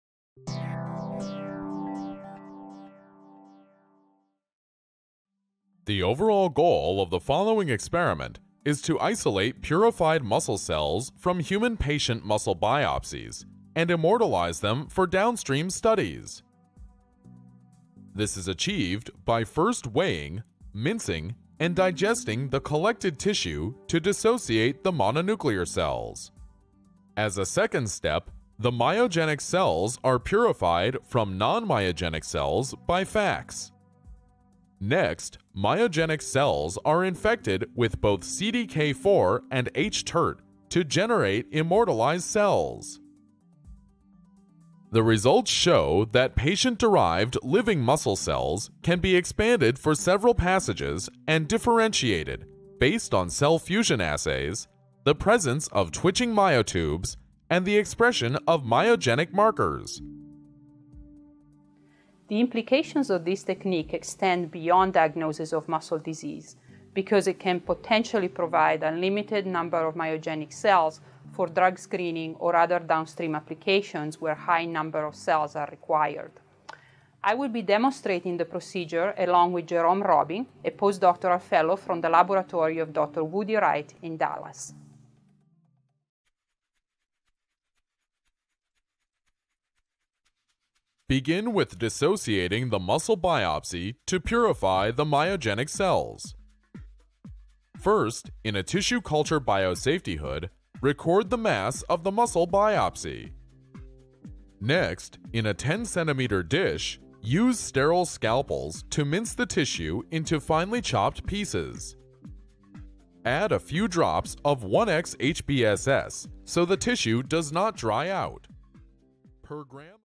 Disease Modeling by Immortalization of Cell Lines from Muscle Biopsy | Protocol Preview
Isolation and Immortalization of Patient-derived Cell Lines from Muscle Biopsy for Disease Modeling -  a 2 minute Preview of the Experimental Protocol

Jerome D. Robin, Woody E. Wright, Yaqun Zou, Stacy A. Cossette, Michael W. Lawlor, Emanuela Gussoni
UT Southwestern Medical Center, Department of Cell Biology; National Institute of Health, National Institute of Neurological Disorders and Stroke; Medical College of Wisconsin, Division of Pediatric Pathology, Department of Pathology and Laboratory Medicine; Boston Children&#39;s Hospital, Division of Genetics and Genomics;

This protocol describes techniques for live cell isolation and primary culture of myogenic and fibroblast cell lines from muscle or skin tissue.  A technique for the immortalization of these cell lines is also described.  Altogether, these protocols provide a reliable tool to generate and preserve patient-derived cells for downstream applications.